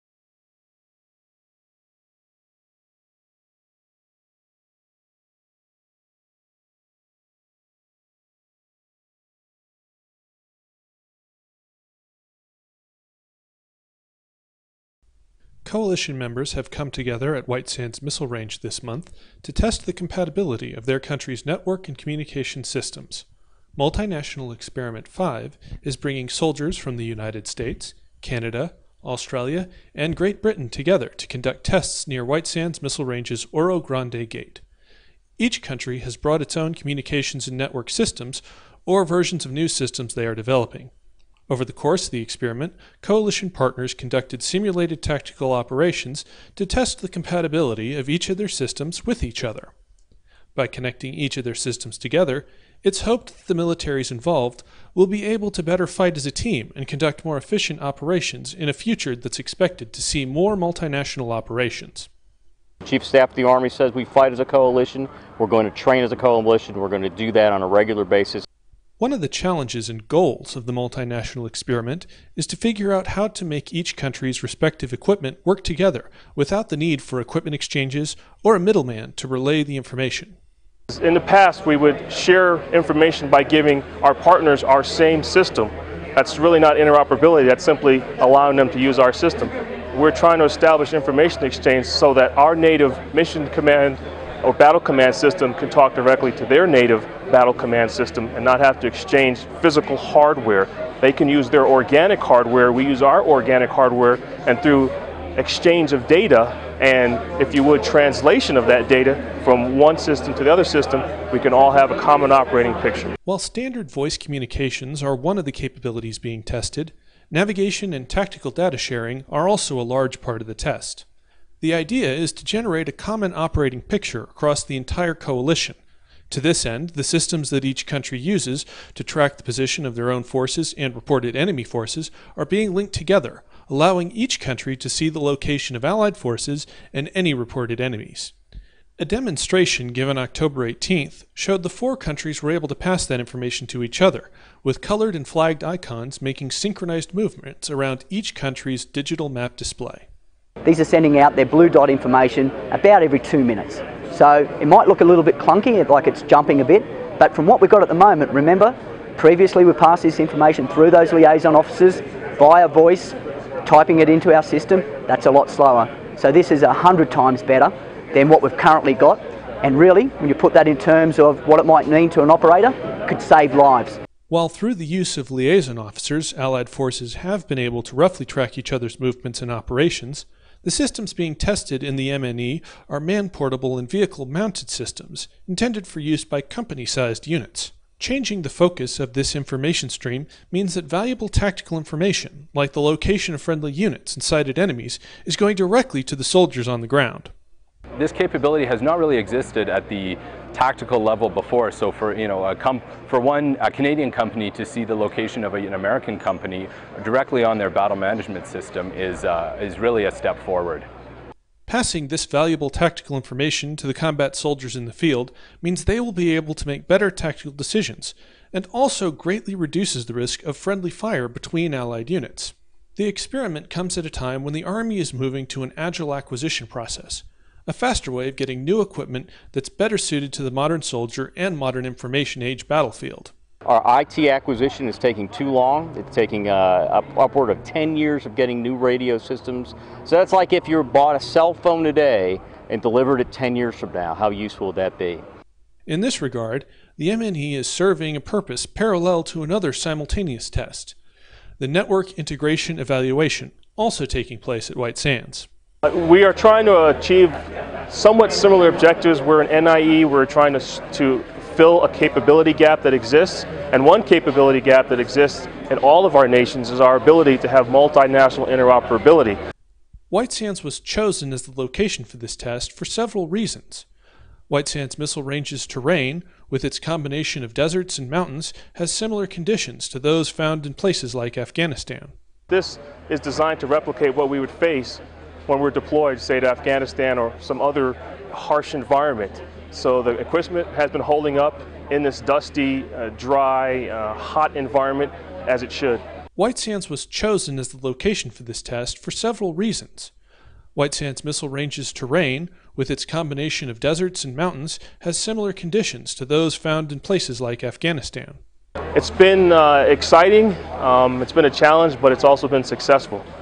Network Integration Experiment 5 (Coalition Forces) Operates at White Sands Missile Range
Coalition members have come together at White Sands Missile Range this month to test the compatibility of their countries' network and communications systems. Multinational Experiment 5 is bringing Soldiers from the United States, Canada, Australia, and Great Britain together to conduct tests near WSMR's Orogrande gate. Each country has brought its own communications and networks systems, or versions of new systems they are developing. Over the course of the experiment, coalition partners conducted simulated tactical operations to test the compatibility of each of their systems with each other. By connecting each of their systems together, it's hoped that the military involved will be able to fight better as a team and conduct more efficient operations in a future that's expected to see more multinational operations.